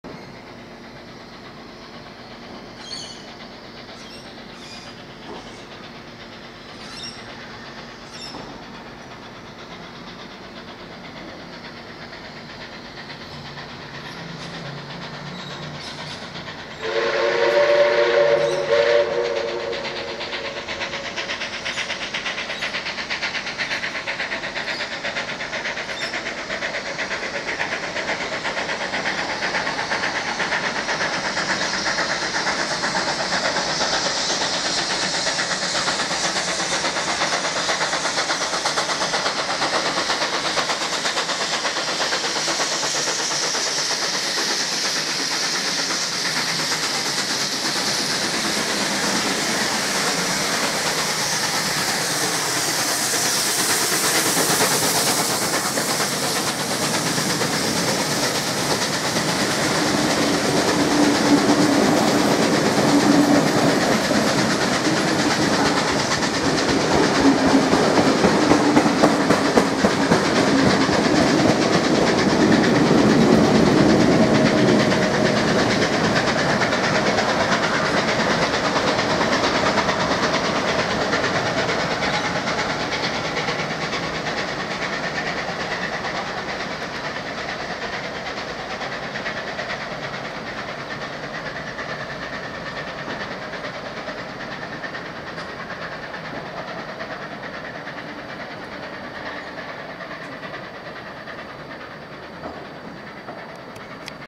5917(LVR) + train + 4204 at Arncliffe 7-11-20
SRS Highlands Picnic Train
5917 (LVR) makes excellent progress for its 8 carriage consist and with 4204 (LVR) attached at the rear as it passes through Arncliffe.